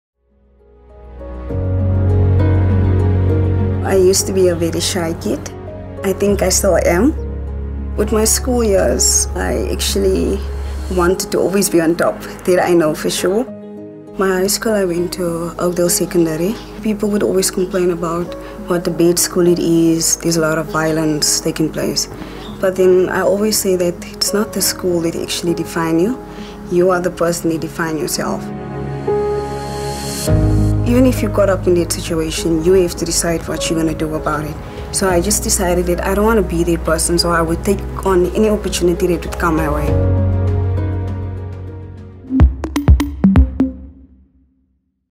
Absa Cybersecurity Month: Melanie Titties
She didn’t let her circumstances dictate her future. Watch Melanie as her message of hope is one that we all need to aspire towards.